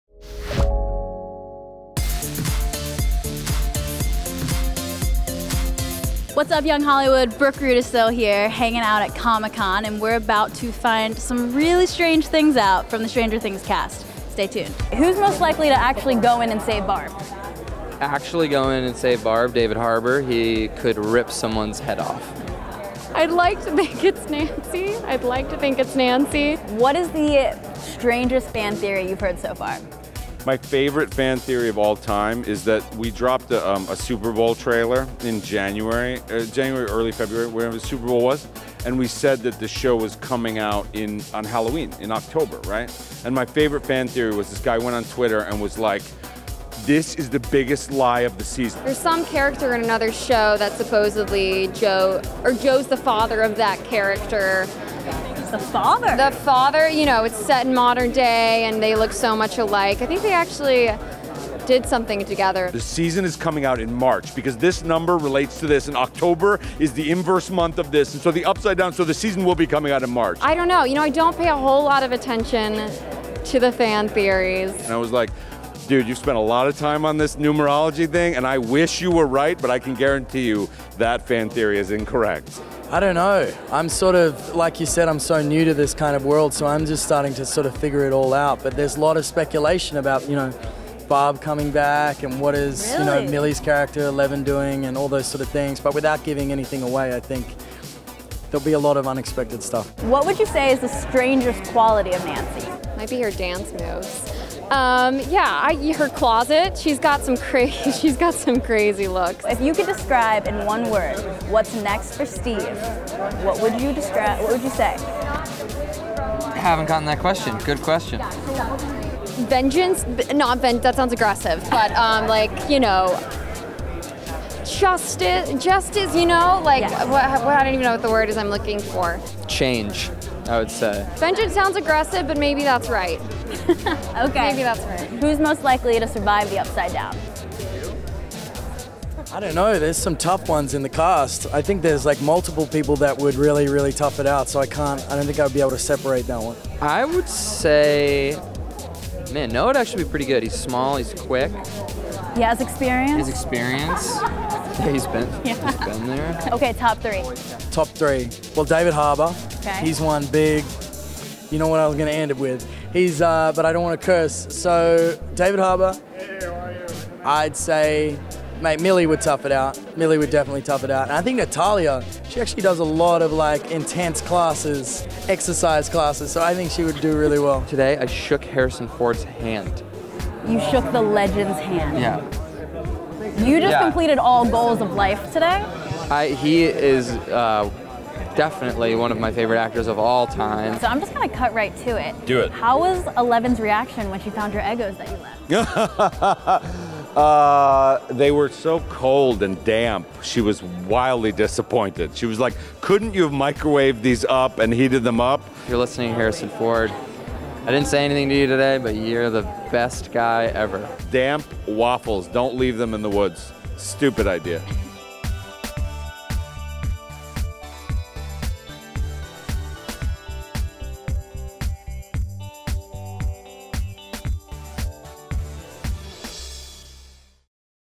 Stranger Things Cast: Millie Could Survive the Upside Down!
The Upside Down comes to San Diego as "Stranger Things" stars Joe Keery, Natalia Dyer, David Harbour, and Season 2 newcomer Dacre Montgomery descend upon Comic-Con! Find out who the actors think would be most likely to save Barb, their characters' strangest quality, and their favorite fan theories, plus which one of them got to shake Harrison Ford's hand!

Apps

2&keywords=young+hollywood

Social

Channels